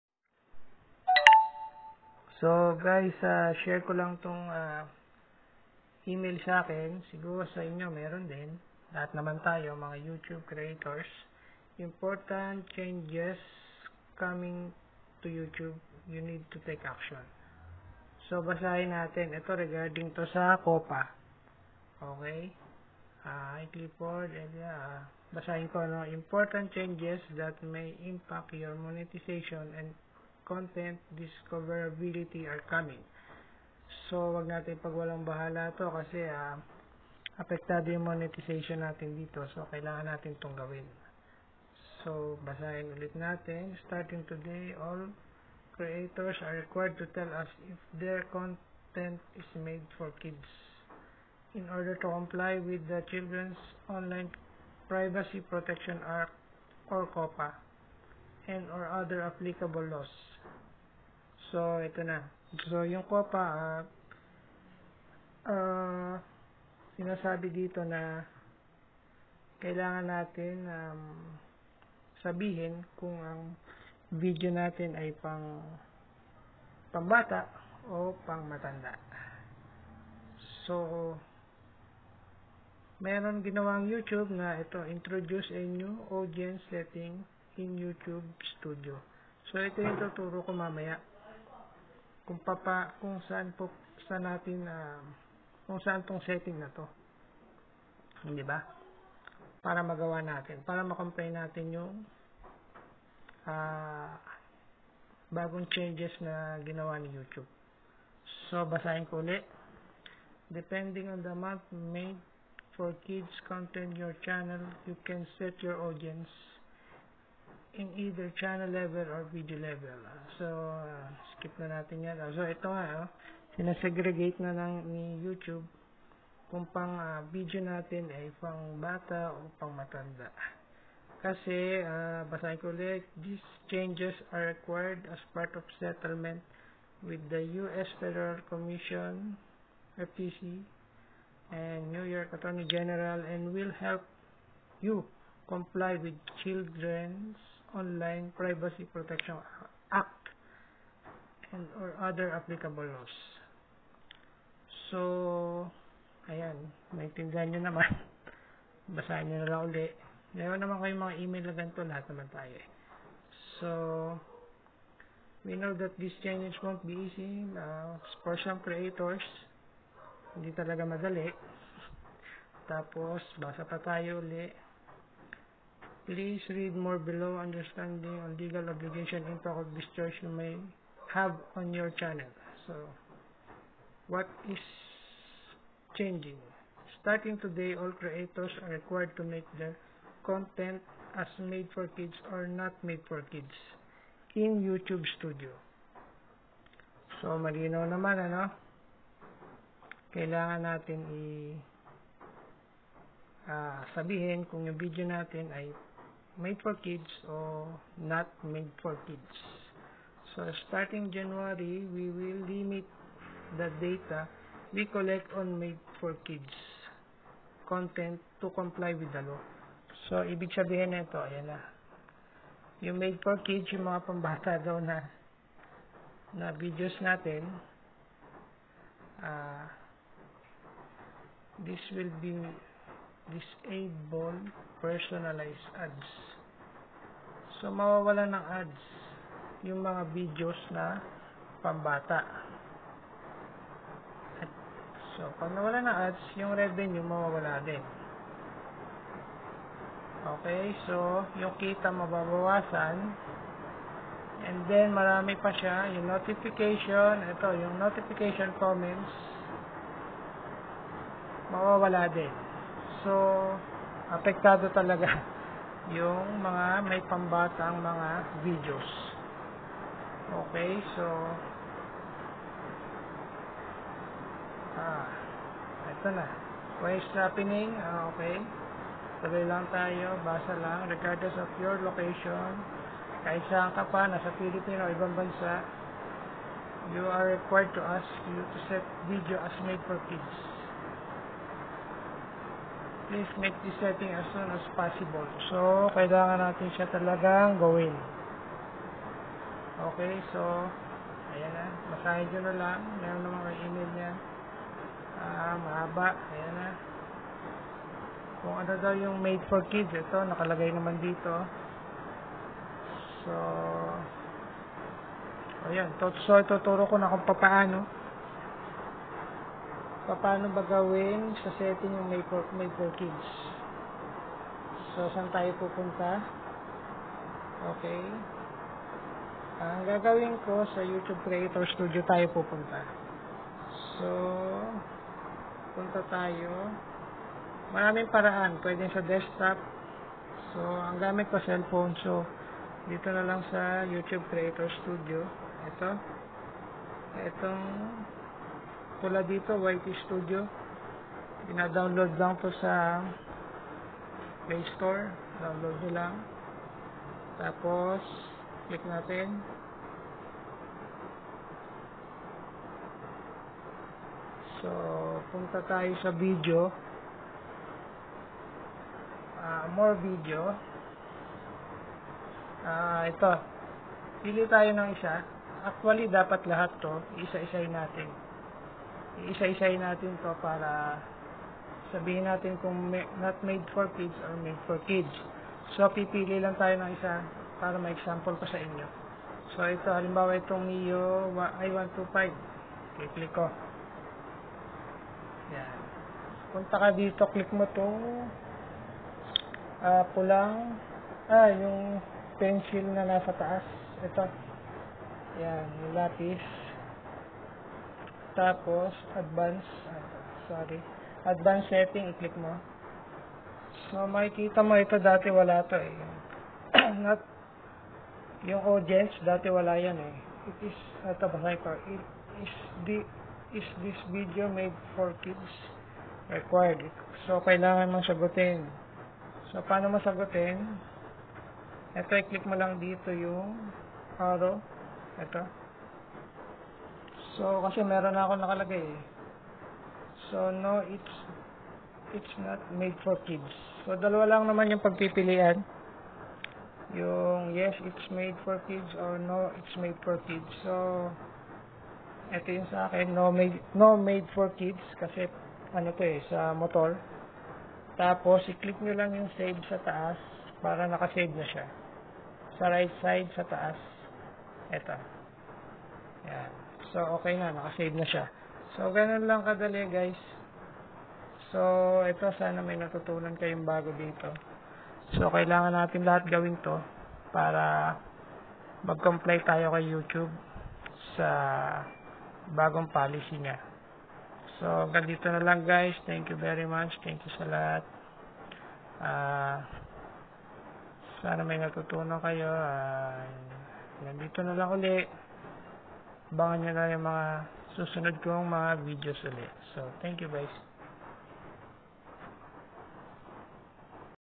COPPA law l new YouTube policy l mobile setting tutorial (tagalog)
Using Oppo A3S android phone